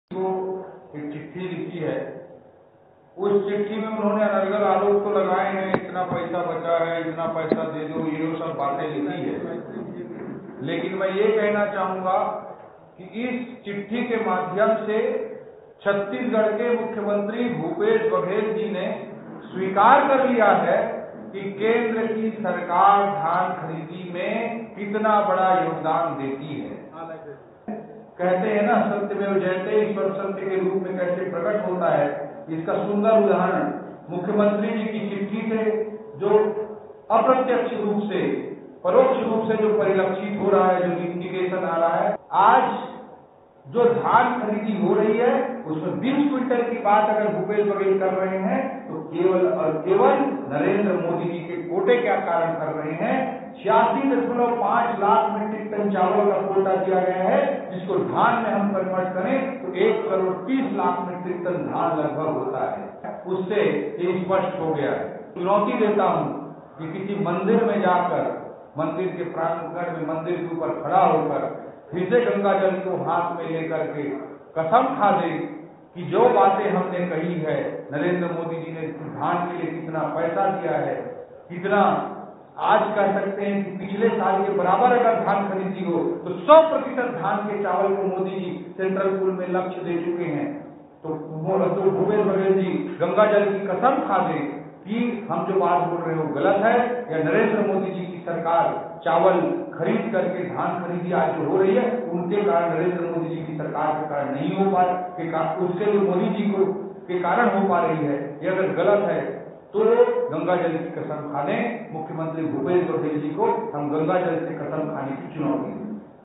केंद्र देता है Chhattisgarh में धान खरीदी का पैसा, CM की चिट्ठी इस बात का प्रमाण | BJP on Congress
Politics On CM Bhupesh Letter : Chhattisgarh में CM Bhupesh Baghel की पीएम मोदी के नाम लिखी चिट्ठी को लेकर सियासत तेज हो गई है. प्रदेश महामंत्री OP Chowdhury ने सीएम भूपेश की चिट्ठी को लेकर सवाल उठाए हैं. ओपी चौधरी ने पूछा है कि चिट्टी में जिस 6 हजार करोड़ बकाया की मांग की जा रही है वो किसलिए है. ओपी चौधरी की माने तो ये पैसा केंद्र का है जो छत्तीसगढ़ के सेंट्रल पूल से धान लेने पर दिया जाता है. लेकिन राज्य सरकार इस पैसे को खुद का बताकर किसानों को गुमराह कर रही है.